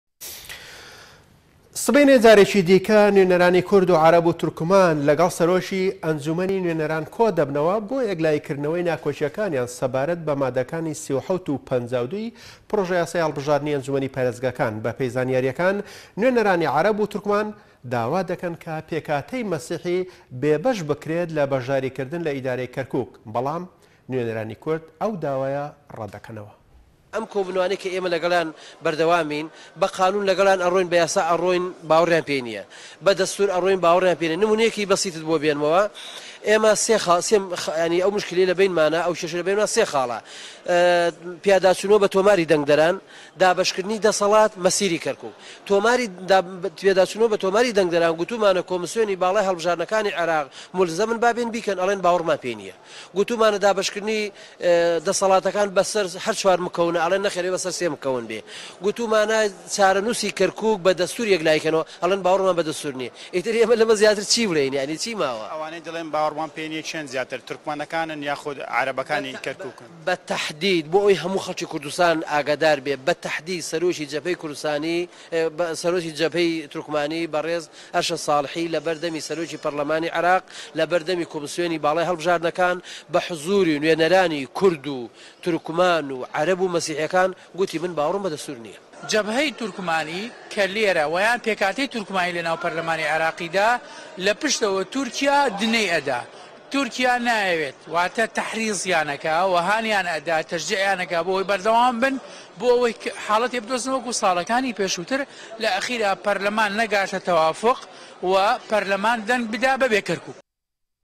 به‌غداد: نوێنه‌رانی‌ توركمان و عه‌ره‌ب داواده‌كه‌ن پێكهاته‌ی‌ مه‌سیحی‌ بێبه‌شبكرێت
سبه‌ینێ جارێكی‌ تر نوێنه‌رانی‌ كورد و عه‌ره‌ب و توركمان له‌گه‌ڵ سه‌رۆكی‌ ئه‌نجومه‌نی‌ نوێنه‌ران كۆده‌بنه‌وه‌ بۆ یه‌كلاكردنه‌وه‌ی‌ ناكۆكییه‌كانیان سه‌باره‌ت به‌ ماده‌كانی‌ 37 و 52ی‌ پرۆژه‌ یاسای‌ هه‌ڵبژاردنی‌ ئه‌نجومه‌نی‌ پارێزگاكان، به‌پێی‌ زانیارییه‌كان نوێنه‌رانی‌ عه‌ره‌ب و توركمان داواده‌كه‌ن پێكهاته‌ی‌ مه‌سیحی‌ بێبه‌شبكرێت له‌ به‌شداریكردن له‌ ئیداره‌ی‌ كه‌ركوك، به‌ڵام نوێنه‌رانی‌ كورد ئه‌و داوایه‌ ره‌تده‌كه‌نه‌وه‌.